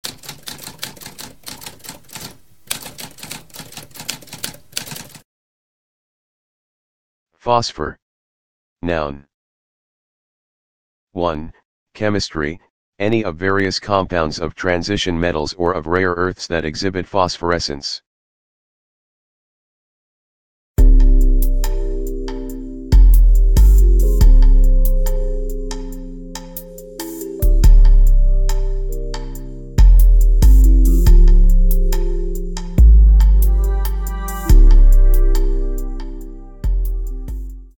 What does phosphor mean?
A spoken definition of phosphor.

Intro Sound:
Typewriter - Tamskp